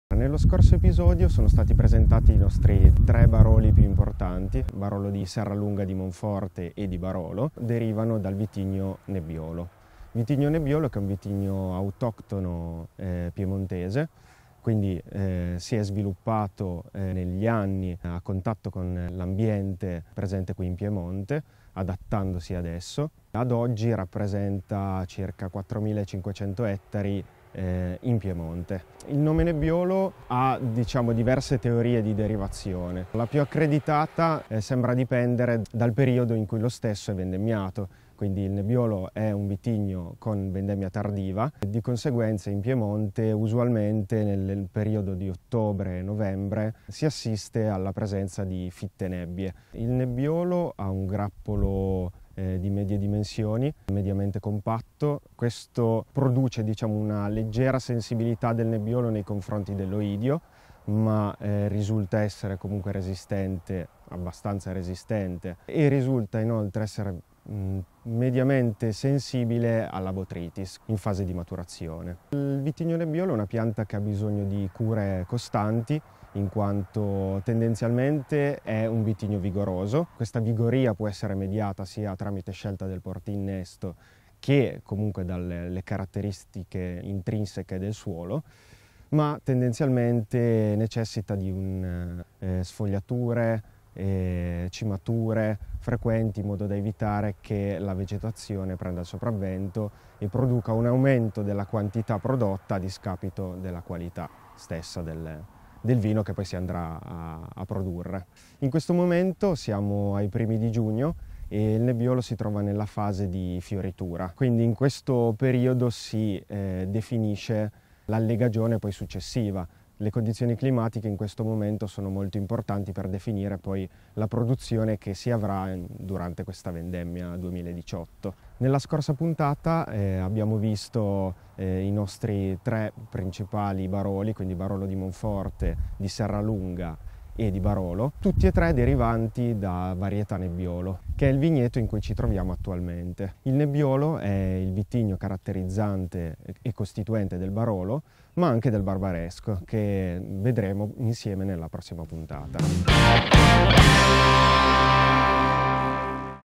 Il vitigno Nebbiolo
Dalla web series ViteColte una video-presentazione del vitigno Nebbiolo piemontese, padre dei Baroli di Monforte, Serralunga e Barolo. Scopriamone insieme le specificità!